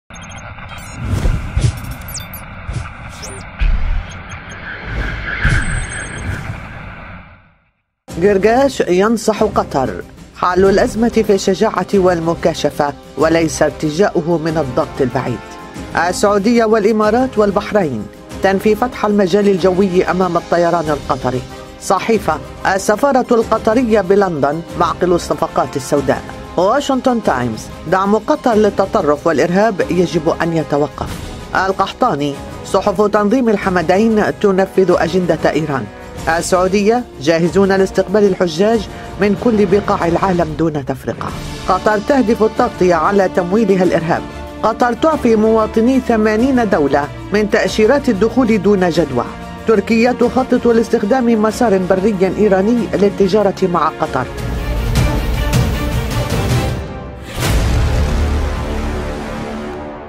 أبرز عناوين ملحق الاتحاد " قطر_تنتحر" اليوم الخميس 10 أغسطس
مونتاج: انس ابوصعب
تعليق : شهرزاد اللمداني